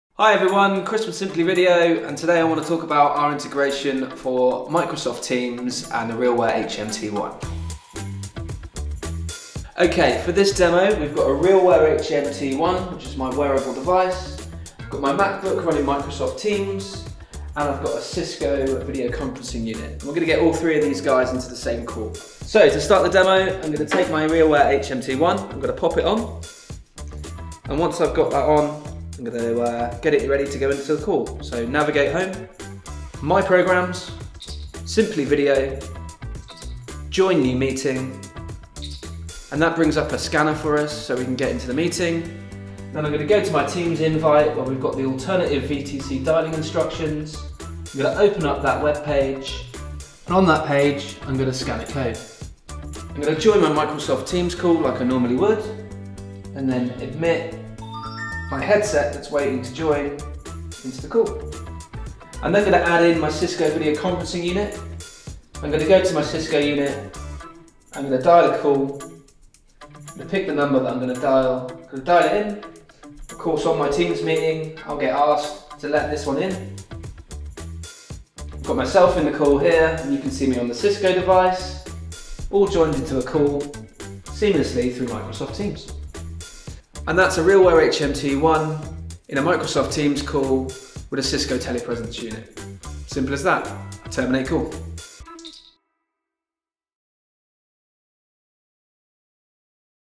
How to: Join a Microsoft Teams call with a RealWear HMT-1 headset.
Ever wondered how you can join a Microsoft Teams call with your RealWear HMT-1 headset?

With SimplyVideo, we've got you covered - watch this simple step by step, and you'll be connected in no time.